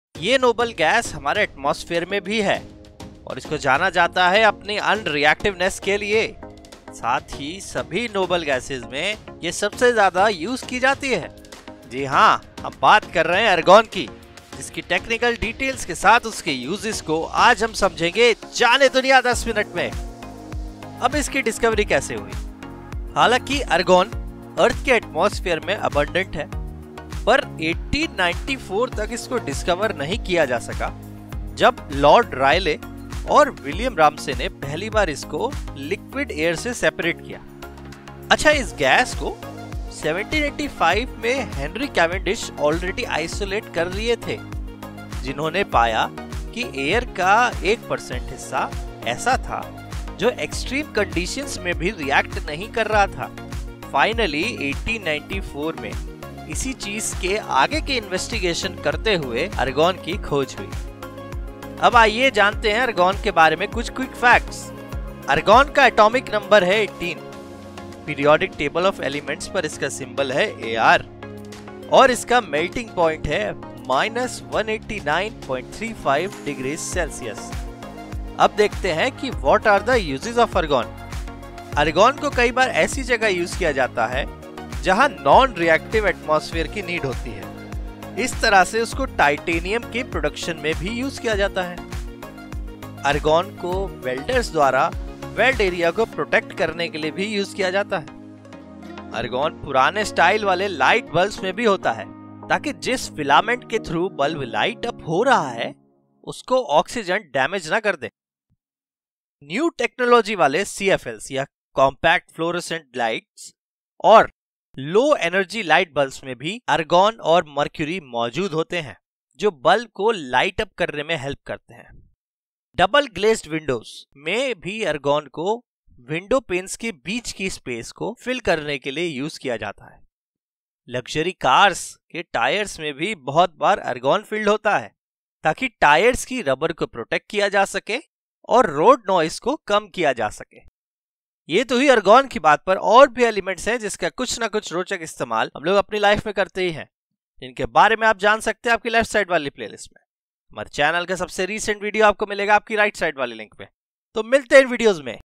Argon Kya Hai | Argon Ke Upyog | Discovery of Argon
Argon Kya Hai | Argon Ke Upyog | Discovery of Argon is explained in this video with full details and other relevant facts.